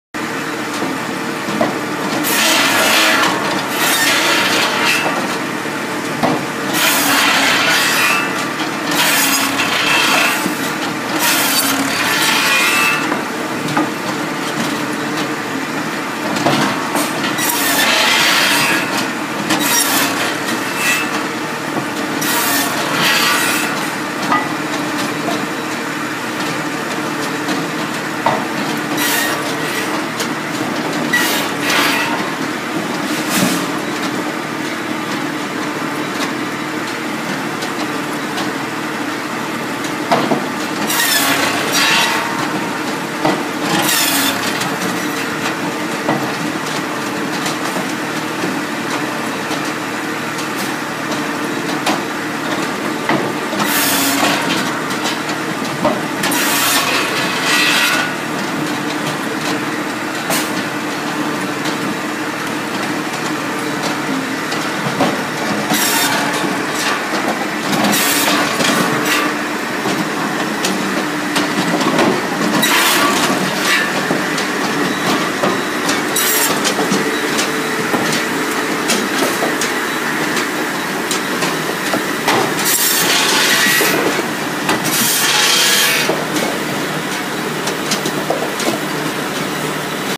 Automatyczne cięcie drewna na pile SmartCut 700 Posch
Pokaz cięcia na automatycznej, grawitacyjnej pile. Więcej na